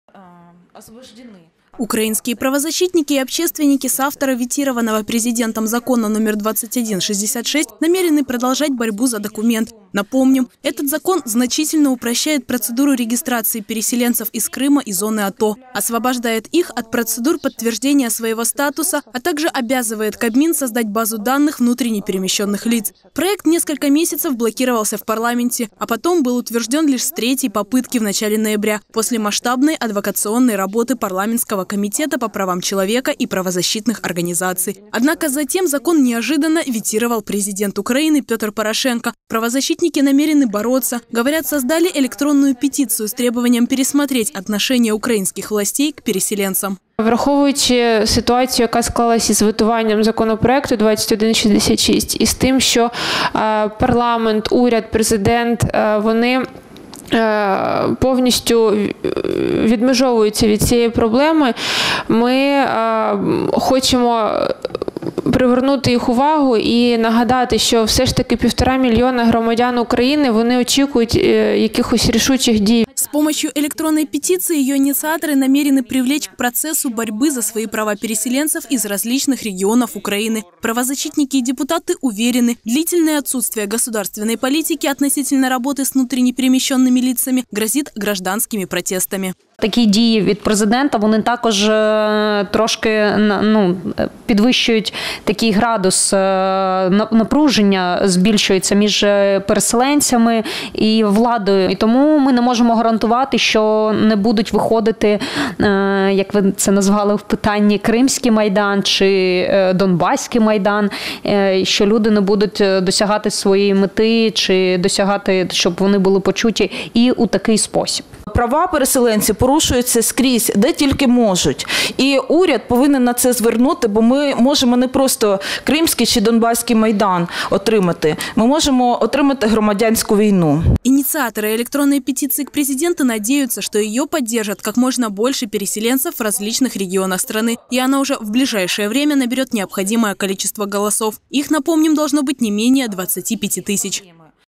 "Переселенцы против президентского вето"
Украинские правозащитники и общественники - соавторы ветированного президентом закона № 2166 - намерены продолжать борьбу за документ. Напомним, этот закон значительно упрощает процедуру регистрации переселенцев из Крыма и зоны АТО, освобождают их от процедур подтверждения своего статуса, а также обязывает Кабмин создать базу данных внутренне перемещенных лиц.
Мы рядом!